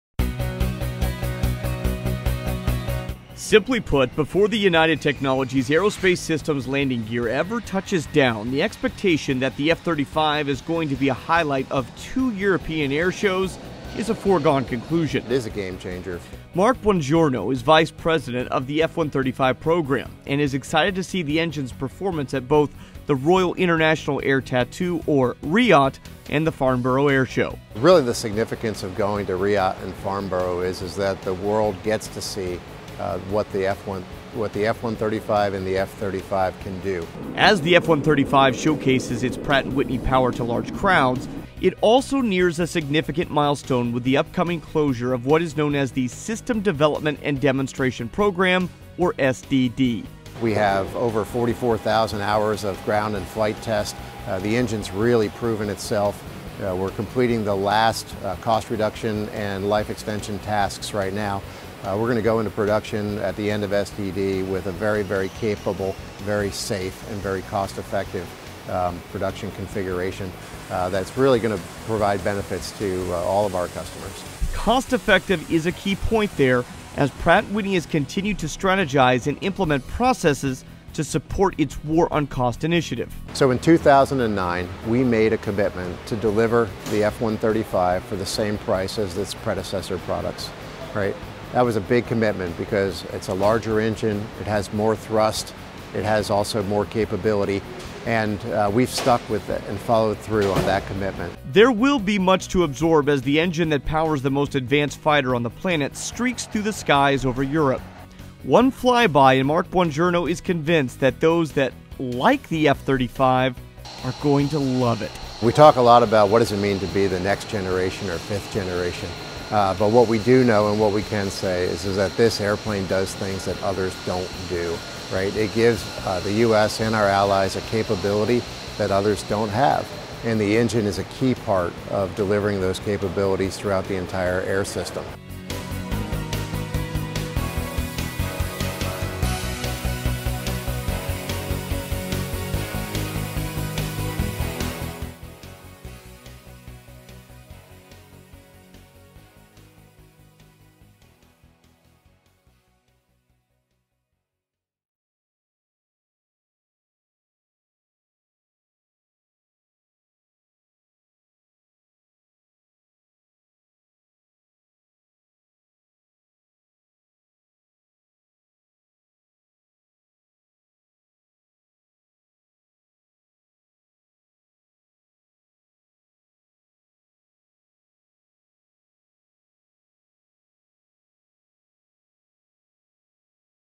F-35, F135 on Display for UK Audience
As the F135 engine showcases its Pratt & Whitney power to large crowds, it also nears a significant milestone with the upcoming closure of what is known as the system development and demonstration program, or SDD.